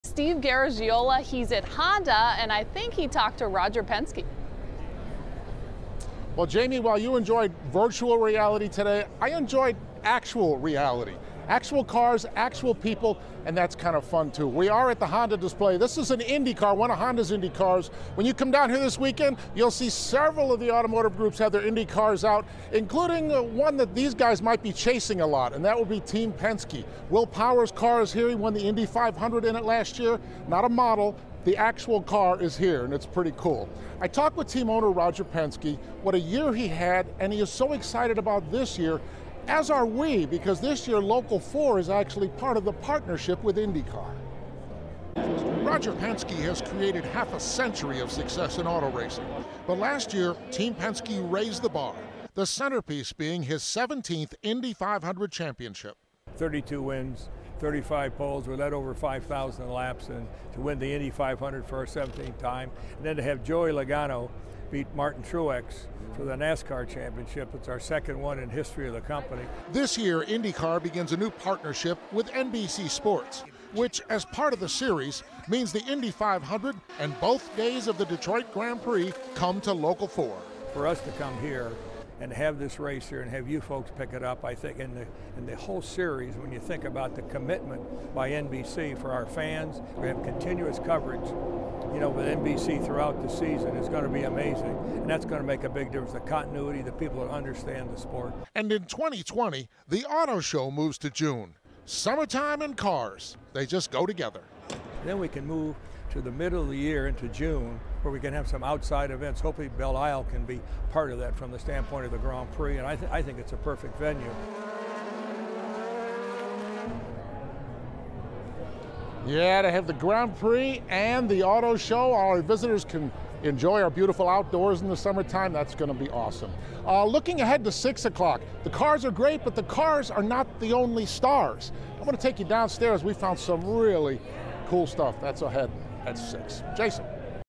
Roger Penske tours show floor at Detroit auto show
Roger Penske toured the show floor Monday at the Detroit auto show.